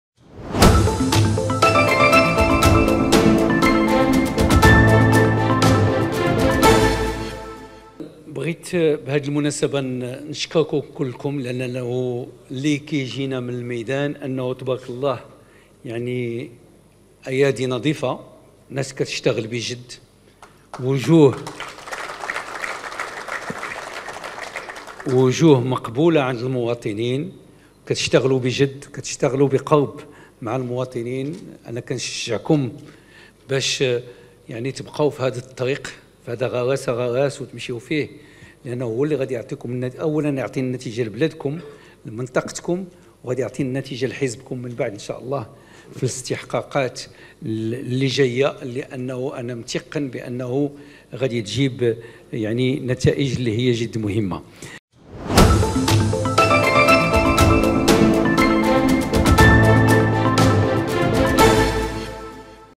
أخنوش: يشجع منتخبي حزبه في جهة الشرق على البقاء في طرق "أغراس أغراس"
طالب عزيز أخنوش رئيس الحكومة أعضاء حزبه في الجهة الشرقية البقاء على  "غراس غراس. وفي هذا الفيديو ستتعرف على بعض ما قاله أخنوش لأعضاء حزبه